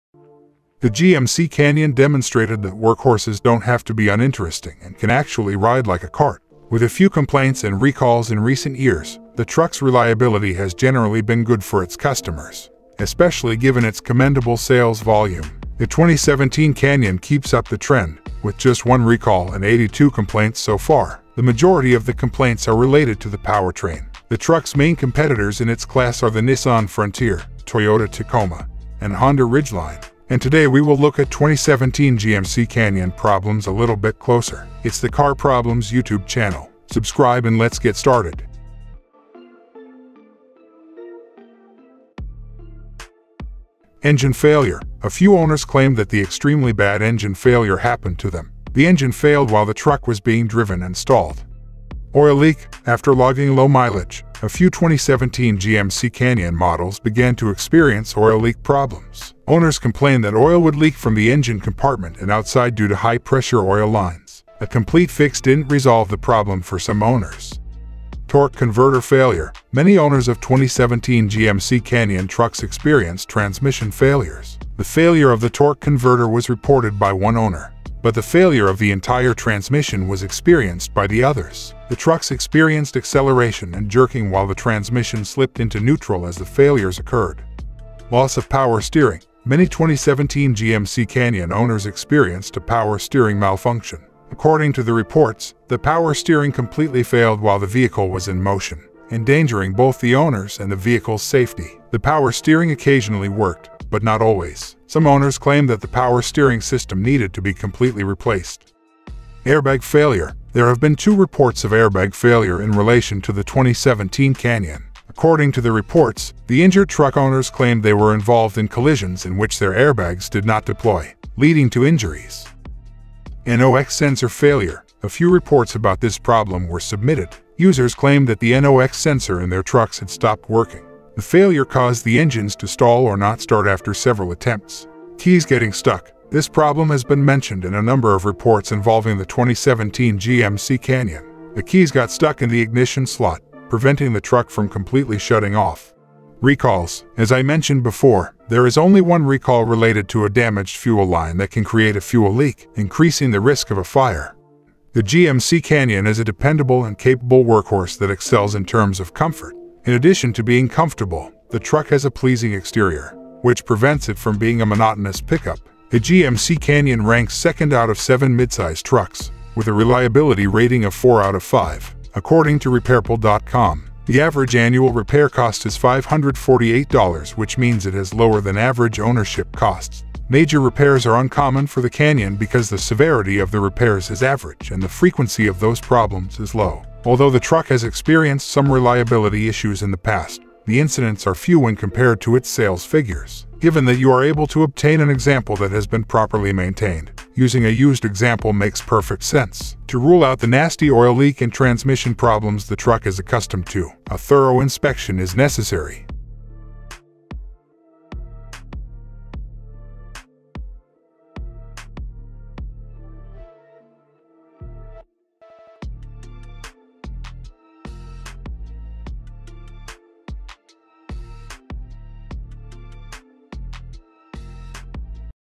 2017 GMC Canyon Problems - Watch this before buy this used truck
The GMC Canyon demonstrated that workhorses don't have to be uninteresting and can actually ride like a cart. With a few complaints and recalls in recent years, the truck's reliability has generally been good for its customers, especially given its commendable sales volume.

Problems list:
00:00:00 - Blah-blah...
00:00:43 - Engine failure
00:00:51 - Oil leak
00:01:08 - Torque converter failure
00:01:27 - Loss of power steering
00:01:49 - Airbag failure
00:02:03 - NOX sensor failure
00:02:14 - Keys getting stuck
00:02:26 - Recalls
00:02:34 - Should yo buy it?